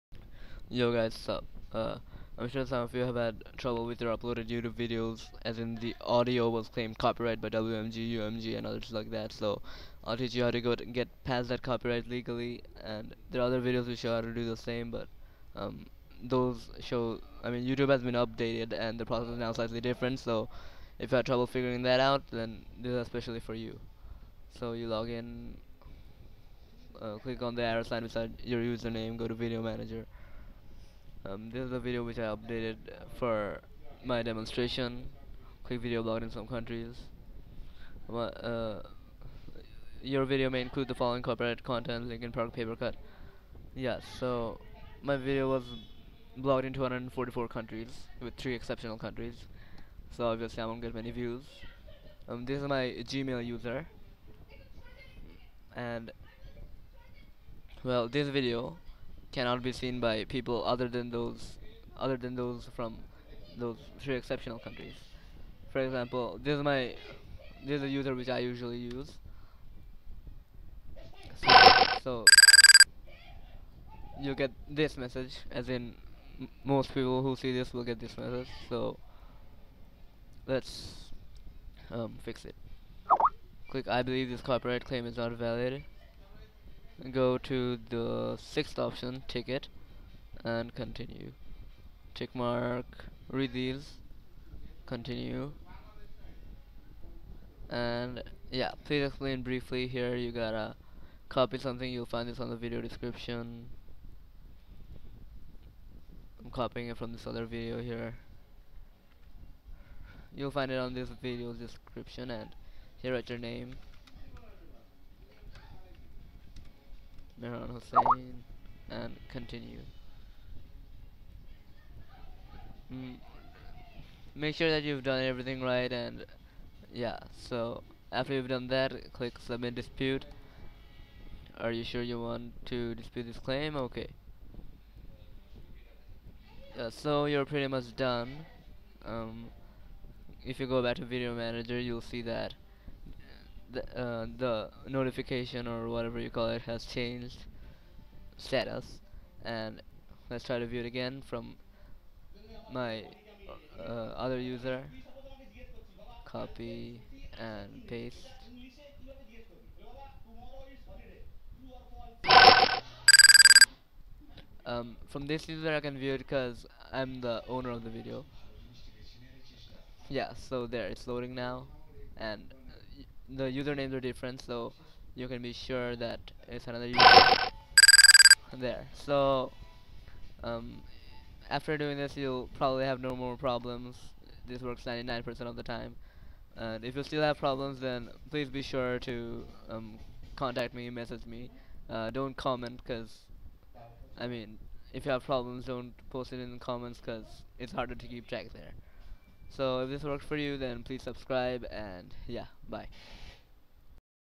How to bypass WMG copyright on youtube (working 2012)
Many video uploaders are having problems with their uploaded youtube videos due to copyrighted audio (not video) claimed by WMG, UMG etc, this video will show how to get past that and get your videos up in less that a minute, and on the updated youtube, but your videos might take a while to be up so you have to wait a while.

Here's the thing that you have to copy in the "please explain briefly" part:
the step by step process:
1. Click on the downward arrow beside your username
2. go to video manager
3. click on the message below the video which had its audio claimed copyright
5. Mark the 5th box and click continue
6. tick the box in the next page and click continue
7. in the "please explain briefly box" copy what written above, tick the box, write your name in the place for your signature and click continue
8. on the next page click submit dispute. You'll be prompted if you are sure you want to submit it or not, be sure to give "ok"
9. You're video should be available very soon (might take longer in unusual cases